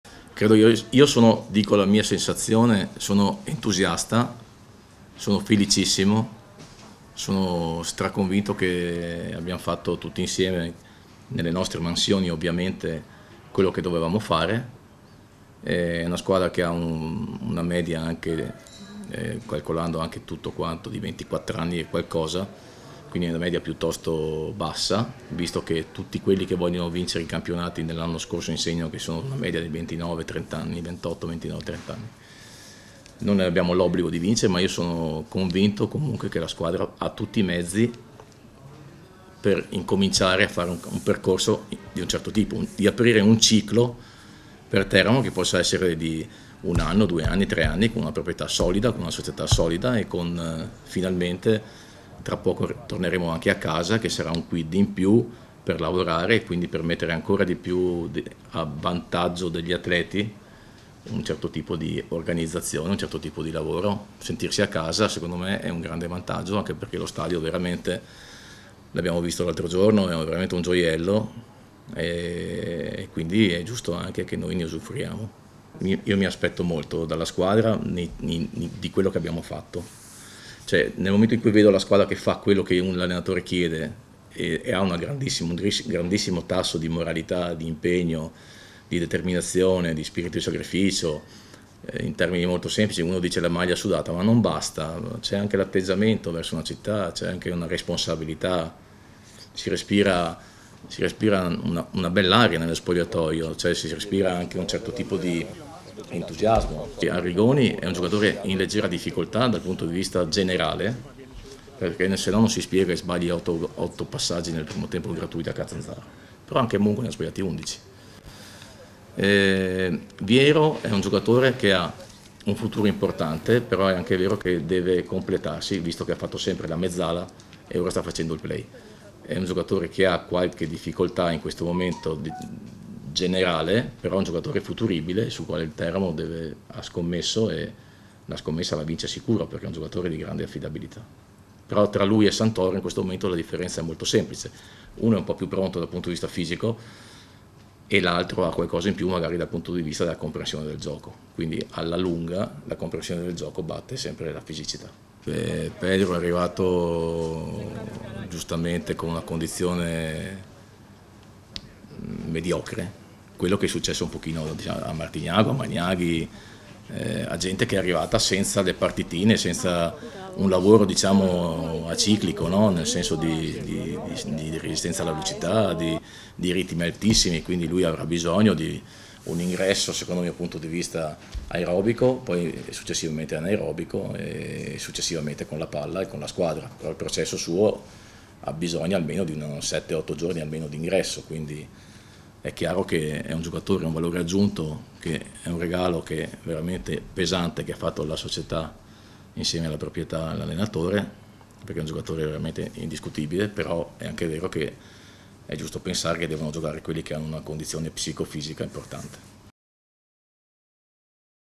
Tedino 'Mi aspetto molto dal Teramo'
Vigilia di campionato per il Teramo dopo la sosta forzata di domenica scorsa. Domani sera biancorossi in campo ad Avellino; buone notizie per Bruno Tedino che recupera Santoro a centrocampo e avrà Cristini in difesa. Probabile coppia d'attacco formata da Magnaghi e Martignago. Il tecnico non avrà a disposizione Minelli, Soprano e l'ultimo arrivato Pedro Costa Ferreira